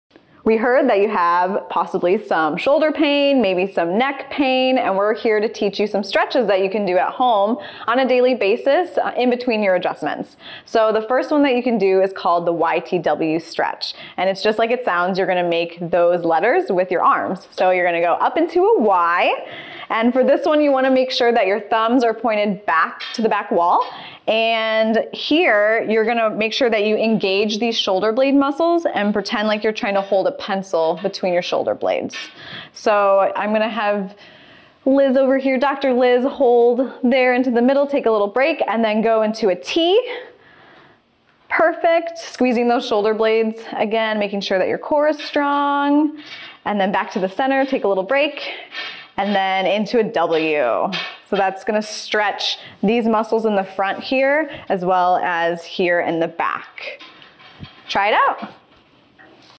Y-T-W Neck Stretch - By The Drs Liz & Lindsay - Chiropractors
Explained in detail, here is a great stretch to do for your neck if you're feeling any pain, discomfort, or have been on the computer/texting for long periods of time. Made by The Drs Liz & Lindsay - Family Chiropractors in Imperial Beach, CA